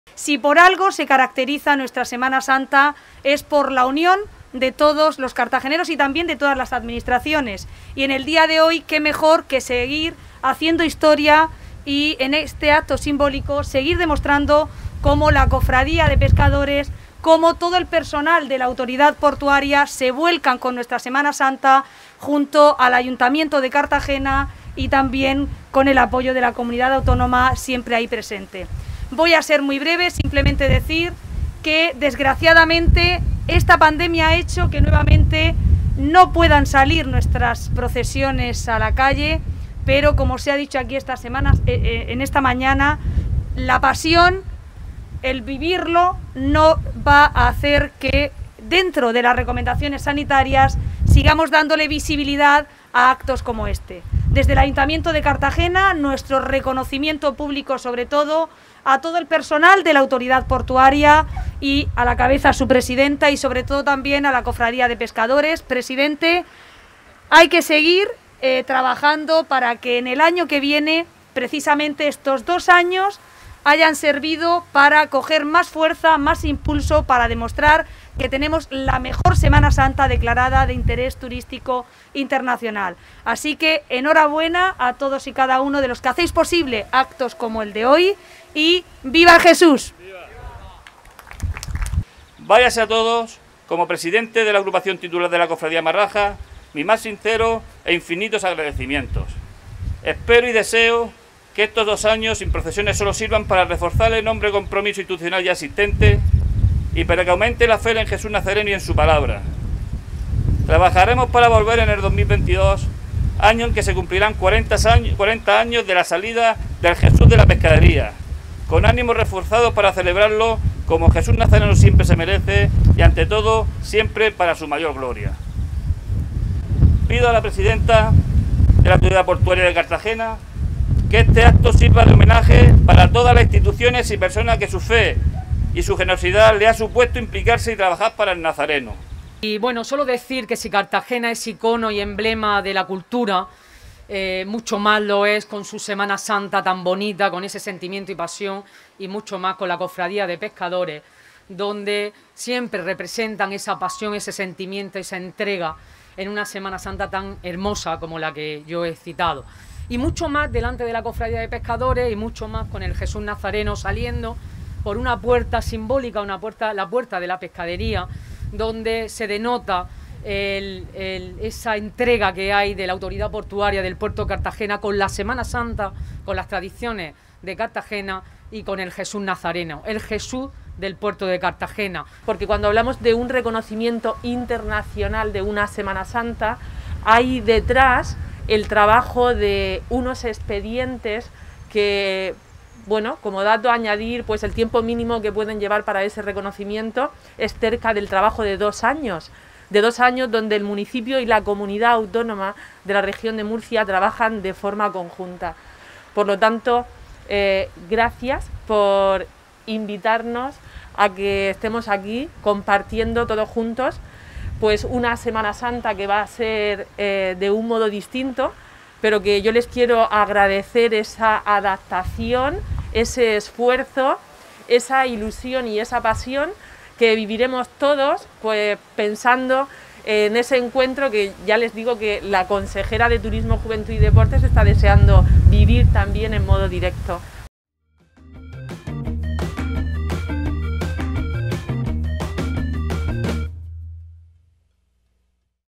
La Agrupación Marraja Nuestro Padre Jesús Nazareno recibe la llave de la Pescadería
La Agrupación Marraja de Nuestro Padre Jesús Nazareno ha recibido esta mañana la llave de la puerta monumental de la Pescadería, cumpliendo así con la tradición cartagenera para la salida del trono del Jesús Nazareno, en la madrugada del Viernes Santo, hacia su Encuentro con la Dolorosa, en la Plaza de la Merced.

Al tradicional acto de entrega han asistido la alcaldesa de Cartagena, Ana Belén Castejón, la vicealcaldesa Noelia Arroyo, y el teniente de alcalde, Manuel Padín, acompañando a la presidenta de la Autoridad Portuaria, Yolanda Muñoz, y al presidente de la Agrupación Marraja Nuestro Padre Jesús Nazareno, Manuel Hernández Aguado, y la consejera de Turismo, Juventud y Deportes de la Comunidad Autónoma de la Región de Murcia, Cristina Sánchez.

En esa colaboración entre el Puerto y la Agrupación Marraja Nuestro Padre Jesús Nazareno, se ha querido homenajear a los trabajadores de la Autoridad Portuaria, a Pepe Segura, Leandro Sánchez, Sebastián Acosta, Damián López y Salvador Martínez, por su entrega, profesionalidad y voluntad para organizar todos los preparativos previos a la salida desde hace 39 años.

La alcaldesa, Ana Belén Castejón, les ha agradecido la labor que llevan a cabo de forma altruista y que contribuye a mantener las tradiciones de nuestro municipio y de nuestra Semana Santa.

Por su parte, Manuel Hernández, ha destacado que “este año no tenía por qué ser diferente. Un acto que muestra de la implicación de Cartagena con su Semana Santa, del Ayuntamiento al lado de las Cofradías y Agrupaciones de nuestra querida ciudad; de la Cofradía de Pescadores y sus gentes y de los trabajadores portuarios para acondicionar la zona para la salida del Jesús desde hace 39 años”.

La presidenta de la APC ha insistido “en este compromiso que tenemos con Cartagena, han sido muchos los trabajadores de la Autoridad Portuaria los que han colaborado con la Cofradía Marraja para la salida del Jesús. Personas comprometidas con el Puerto, con la historia y las tradiciones de Cartagena.”

La Agrupación ha querido también dar un reconocimiento a la presidenta de la Autoridad Portuaria, a la consejera de Turismo, a la alcaldesa de Cartagena, a la vicealcaldesa de Cartagena y al primer teniente alcalde de Cartagena, con un cuadro de nuestro Padre Jesús Nazareno.

La puerta monumental de la Pescadería, fabricada en los talleres de la desaparecida empresa Tamar, se colocó en marzo del año 2.000 con el propósito de evitar que el tercio marrajo tuviera que girar tres veces antes de poner rumbo a Santa Lucía, y es tradición desde entonces que se abra una sola vez al año, para que pase Jesús Nazareno. La Autoridad Portuaria custodia durante todo el año las llaves de esta magnífica puerta, y unos días antes del Viernes Santo, se celebra este acto por el que las llaves quedan en poder del tercio de Jesús Nazareno hasta finalizar la Semana Santa.